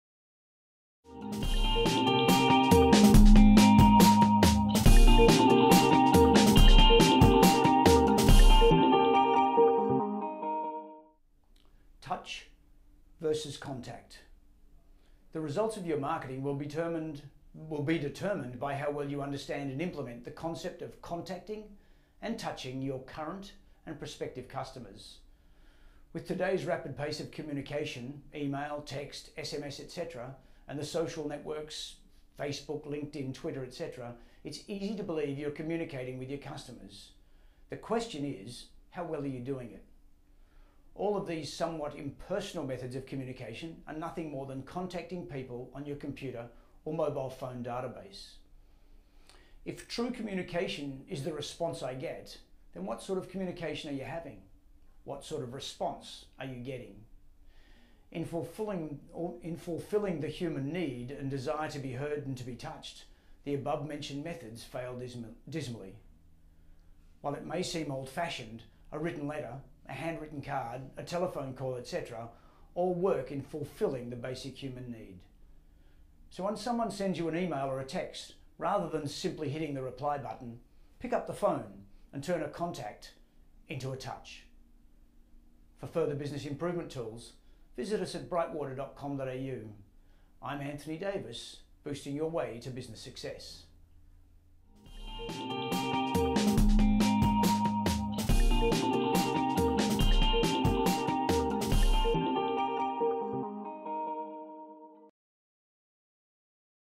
Customer Relationship Management - Brightwater Business Coaching
Customer Relationship Management - Touch vs. Contact

The results of your marketing will be determined by how well you understand and implement the concept of Contacting and Touching your Current & Prospective Customers.

With today's rapid pace of communication ... e-mail, text, SMS etc and the Social Networks, Facebook, LinkedIn, Twitter etc ... it's easy to believe you are communicating with your Customers.  The question is "How well are you doing it?"

All of these somewhat 'impersonal' methods of communication are nothing more than 'contacting' people on your computer or mobile phone database.  If 'true communication' is 'the response I get!' then what sort of communication are you having?  What sort of response are you getting?

In fulfilling the human need and desire to be heard and to be touched the above mentioned methods fail dismally.

While it may seem old fashioned, a written letter, a hand written card, a telephone call etc, all work in fulfilling the basic human need.  So when someone sends an email or a text, rather than simply hitting the reply button ... pick up the phone and turn a 'Contact' into a 'Touch'.